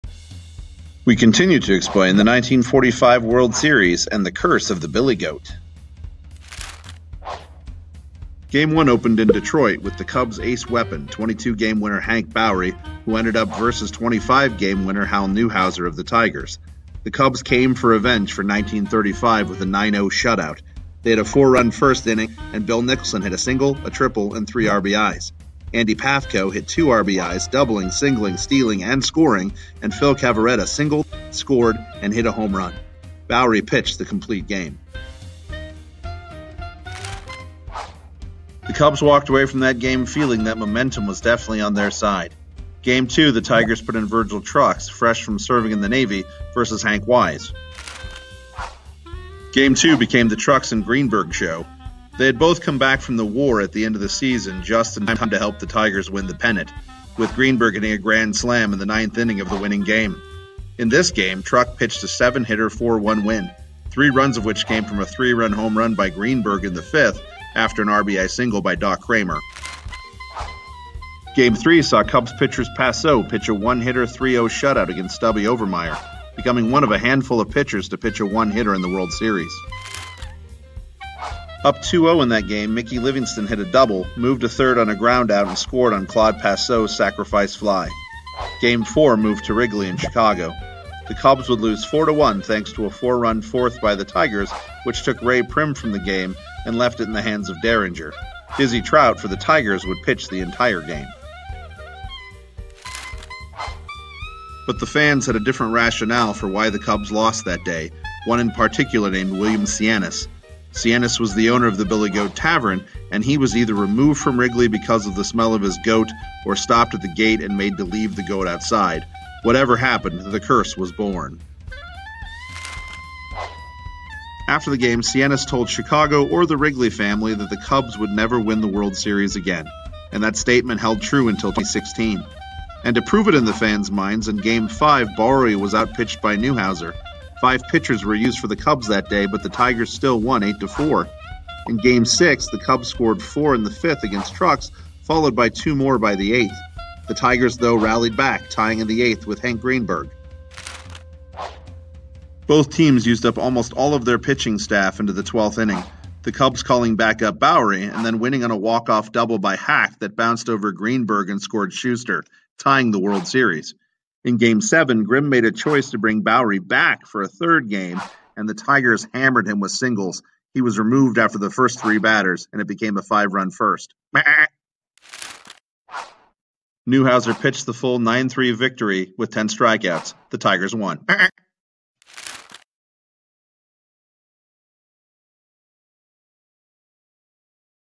1945 World Series B-History of Baseball Class (The Curse of the Billy Goat)
Information gathered from the Baseball Hall of Fame, and Wikipedia.  Images not used with permission but with our apologies and hope for forgiveness, as we are a school project and make 0 financial gain from our product.

 Thank you Music by Pedro for the background music